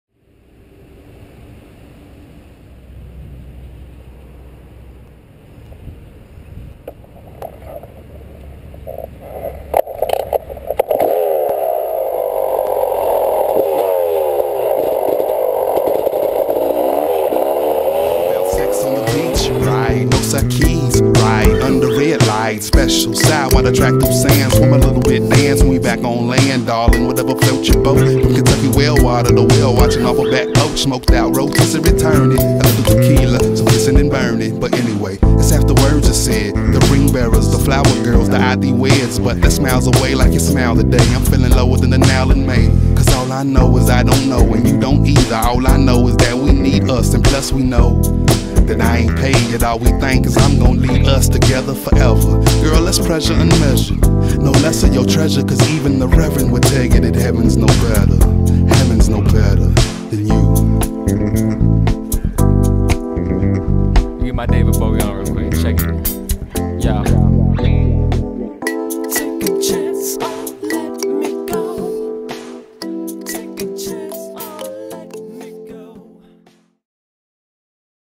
Freestyle Motocross Slow Motion Gopro - Renaud Margry
Some tricks at training with the gopro on the handlebar filming backward! It looks pretty good!